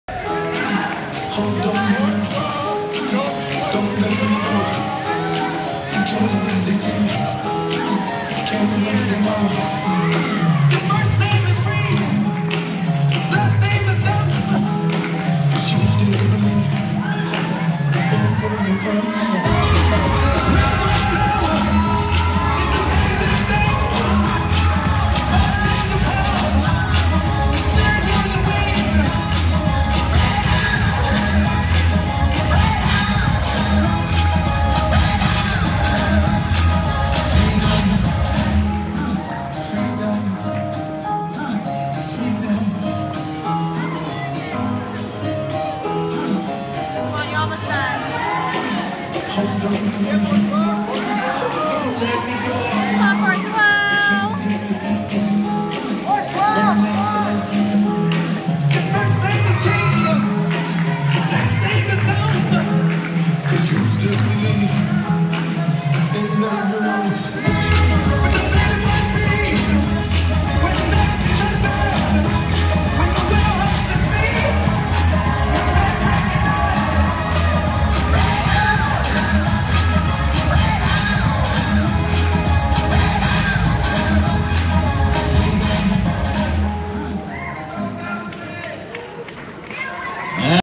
DCDI 2015 Prechamp Quickstep Final
1st place in WTFQ - Tommy Wyderko and Brandy Arredondo